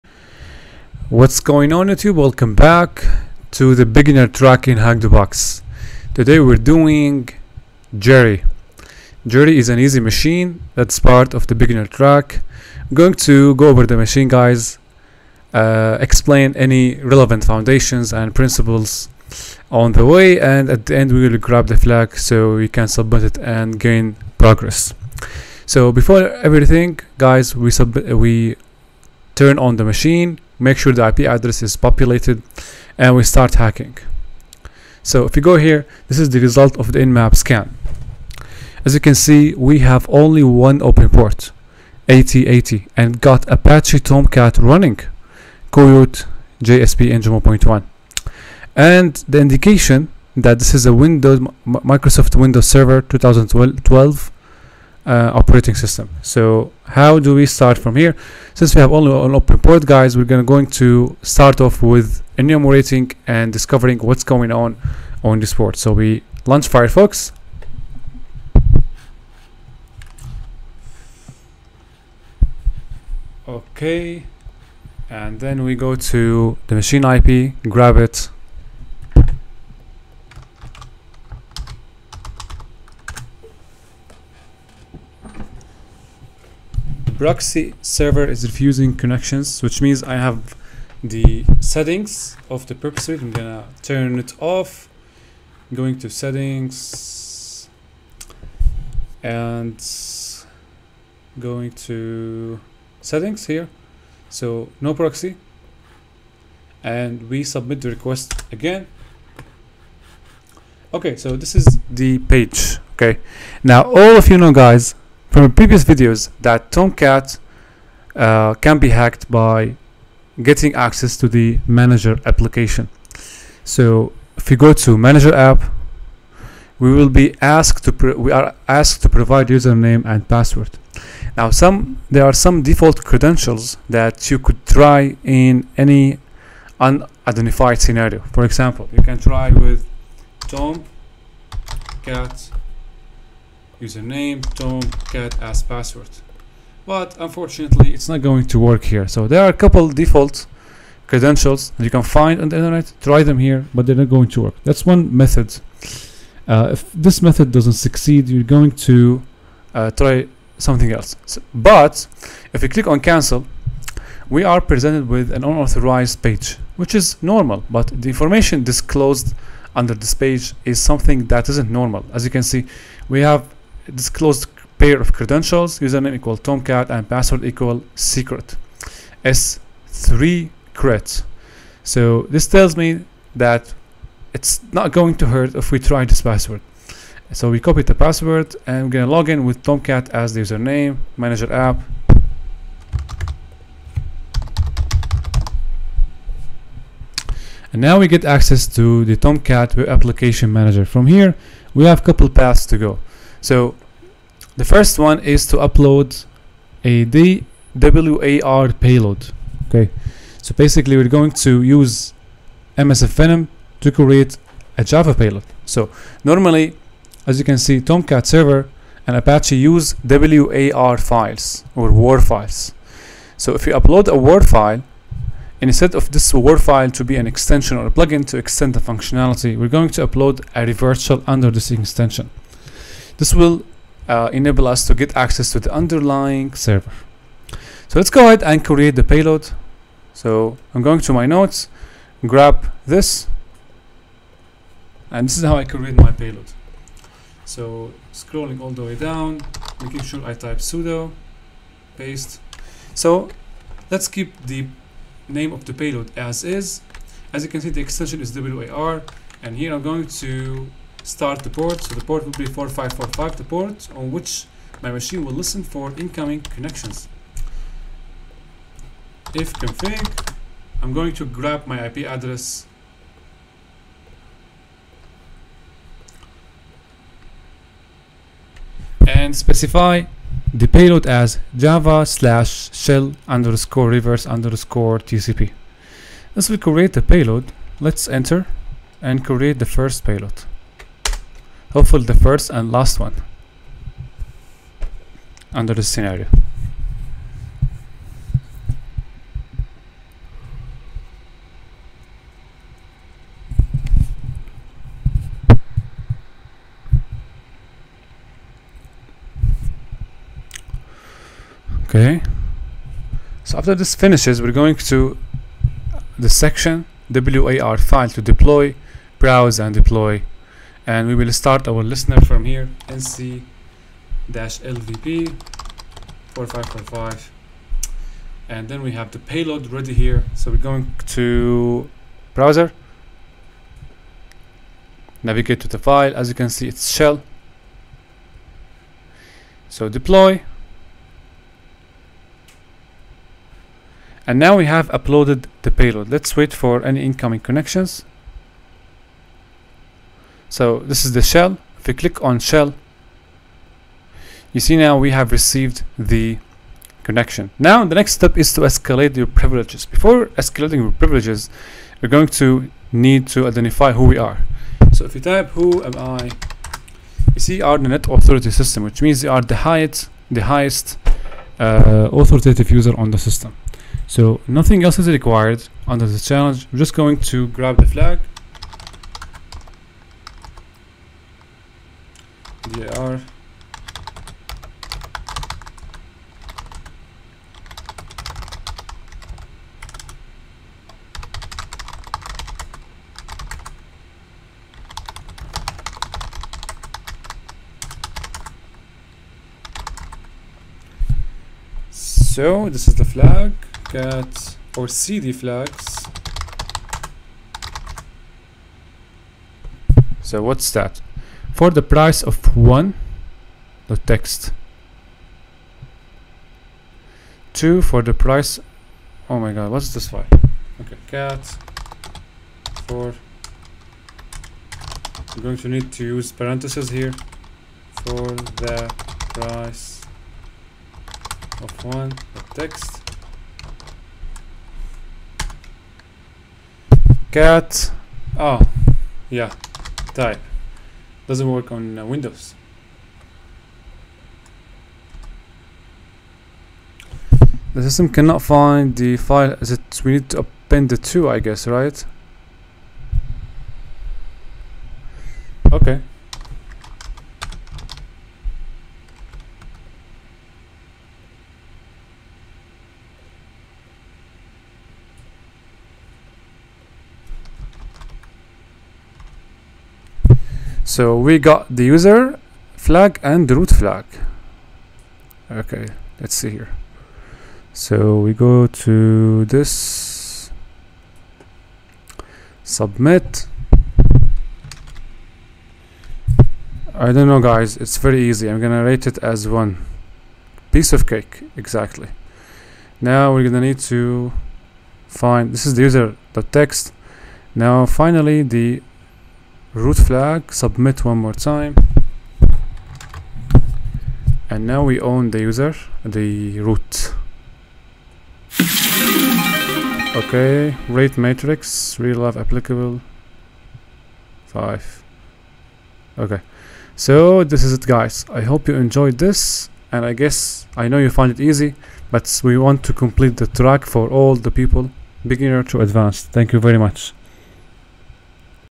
From Apache Tomcat To Shell | CTF Walkthrough
In this video walk-through, we covered HackTheBox Jerry as part of the beginner track in Hackthebox. We demonstrated the move from Apache Tomcat into complete shell access.